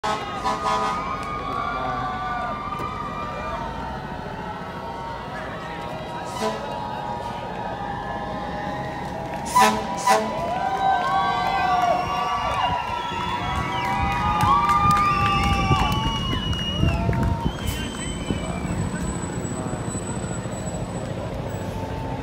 Astro fans Storm the Houston Academy stores World Series 2022
Thousands of people at all the Academys in Houston